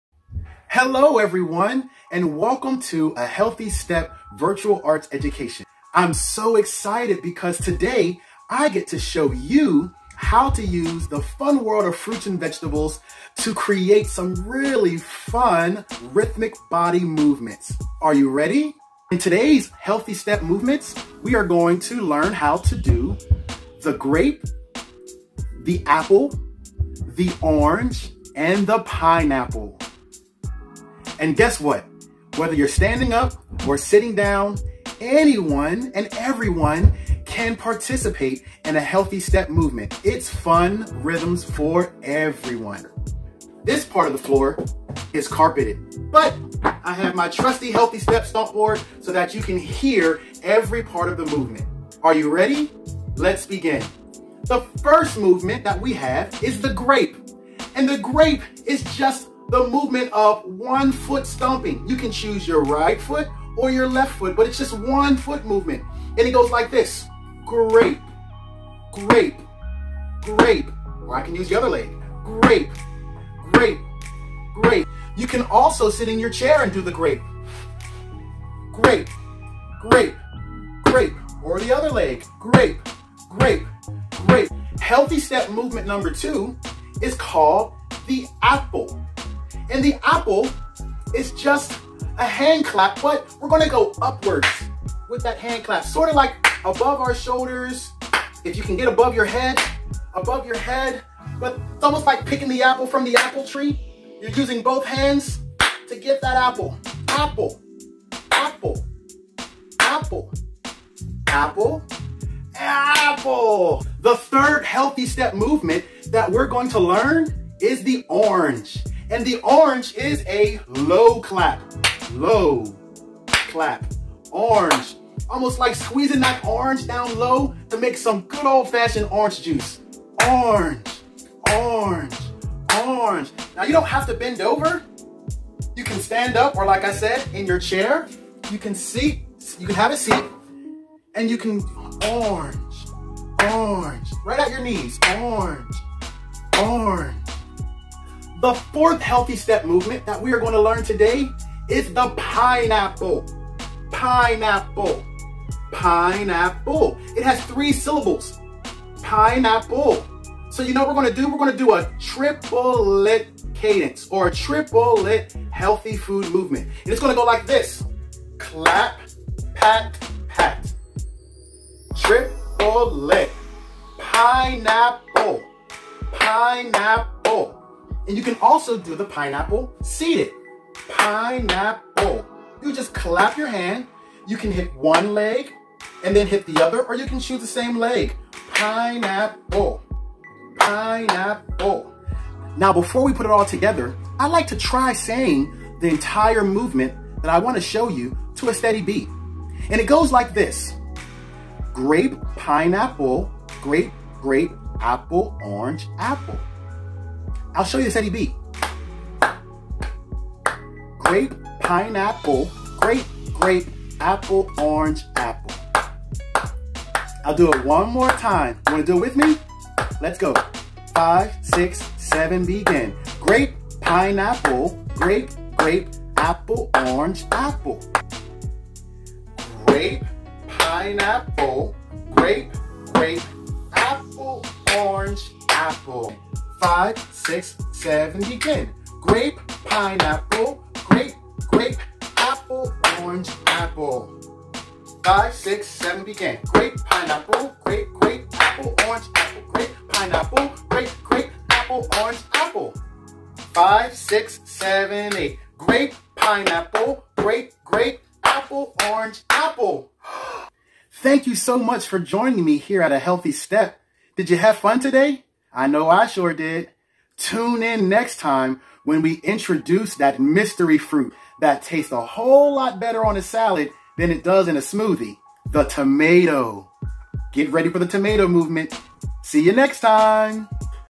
A Healthy Step
Learn fun new dance moves with percussionist Dennis Garner. Join him and take a healthy step while getting familiar with the grape, the apple, the orange, and the pineapple! These moves can be learned and practiced sitting or standing.